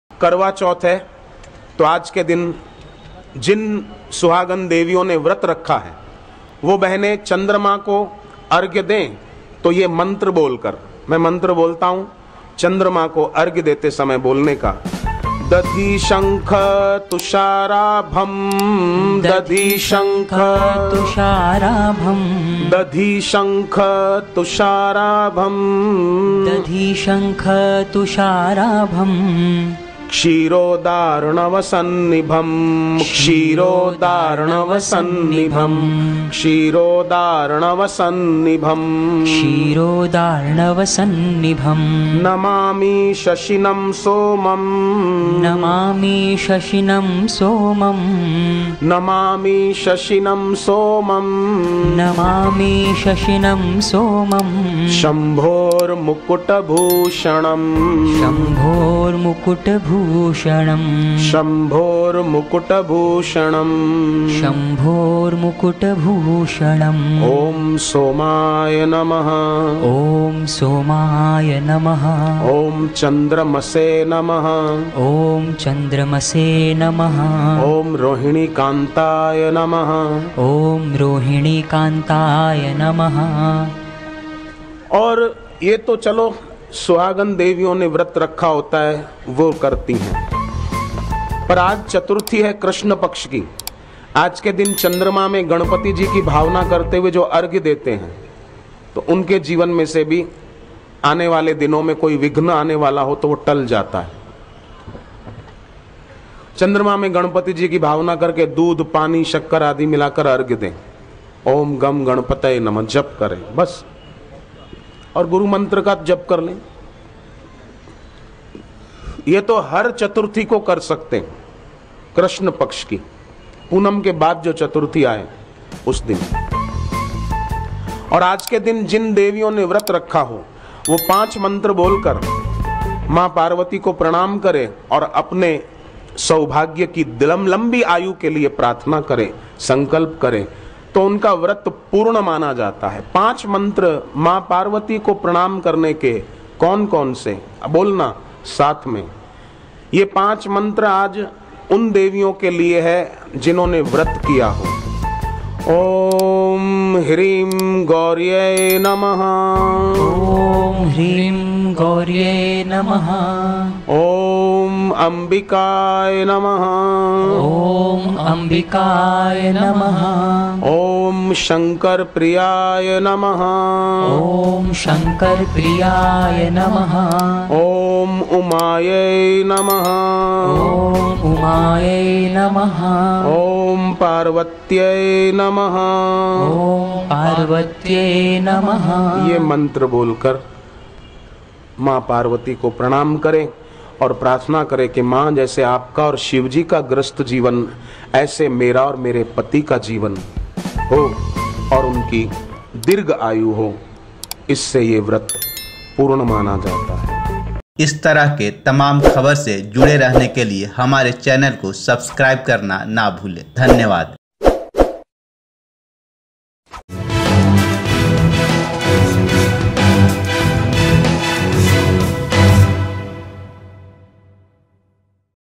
karwa chauth व्रत में जपें ये मंत्र I Karwa chauth Mantra | करवा चौथ का मंत्र | Karva chauth 2021 I
दोगुना होगा पति का प्यार , करवा चौथ व्रत में जपें ये मंत्र , करवा चौथ का मंत्र इस वीडियो में आपको सुनने को मिलेगा .